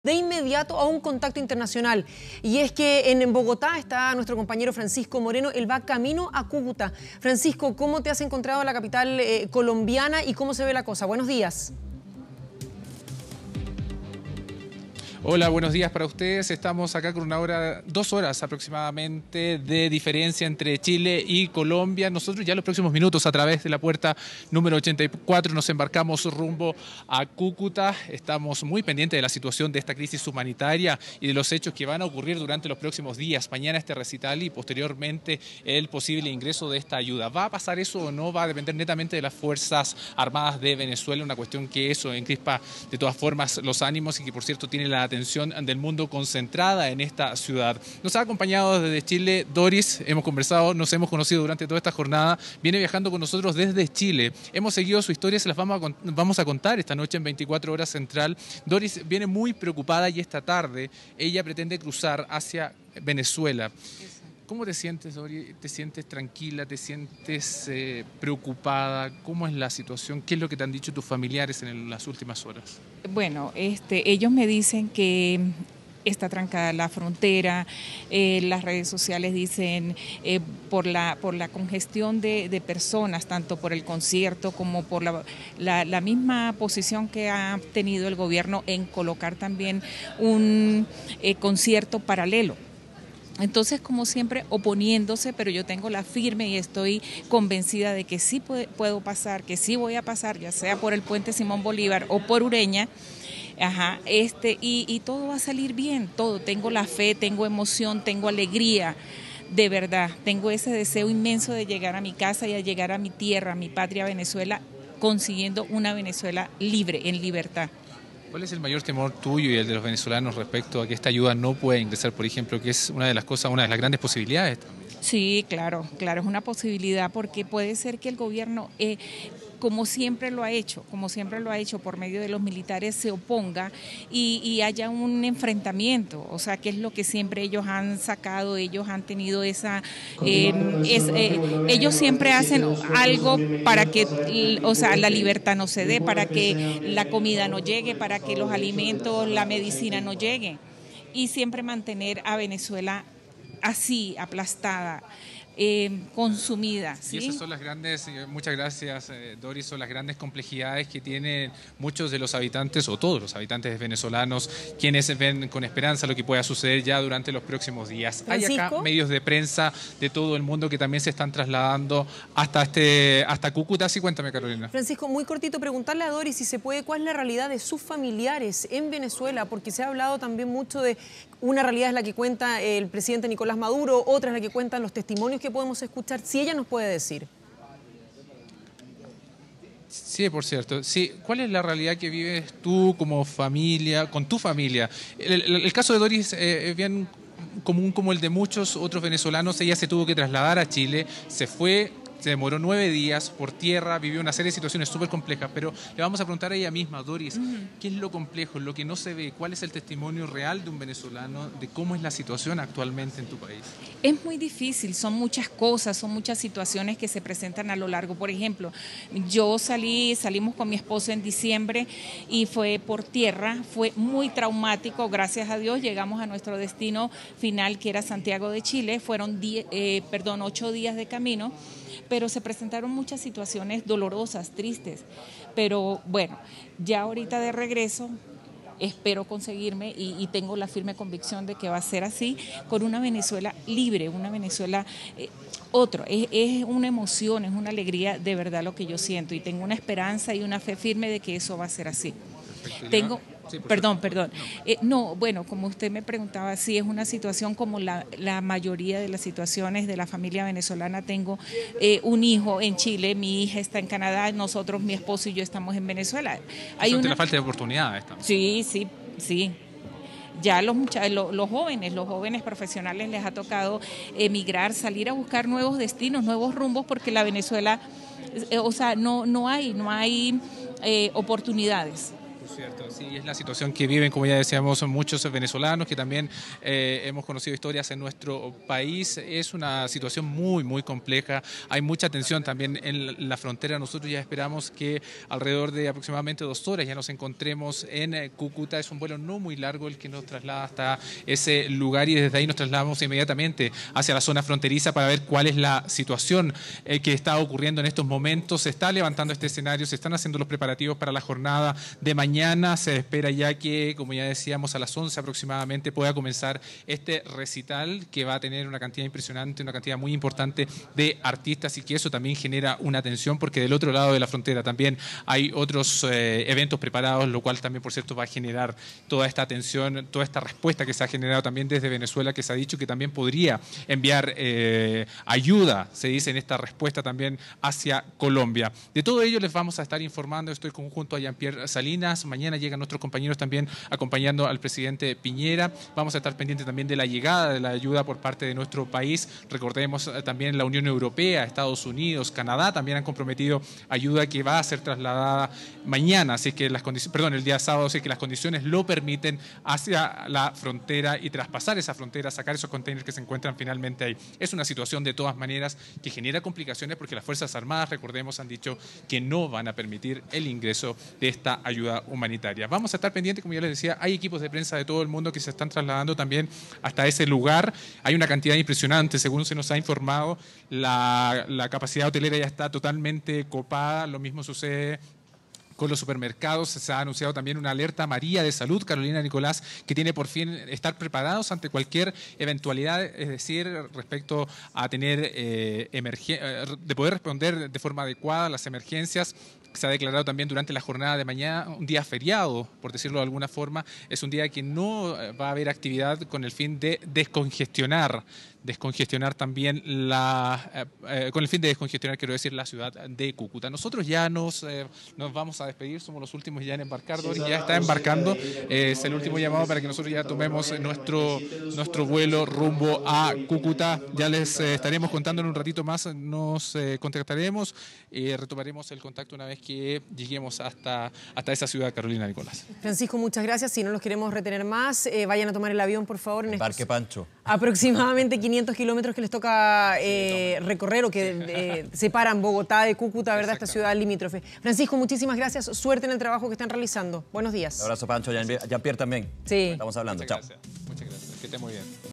Venezolana que vive en Chile retorna a su país: "Tengo fe y emoción de que todo saldrá bien"
Un equipo de 24 Horas se encuentra en Bogotá a la espera de llegar a Cúcuta y presenciar la esperada llegada de ayuda humanitaria. En la previa, el periodista Francisco Moreno conversó con Doris, mujer que se radicó en Chile por la crisis del país llanero, pero ahora pretende cruzar la frontera a las limitaciones puestas por el Ejército.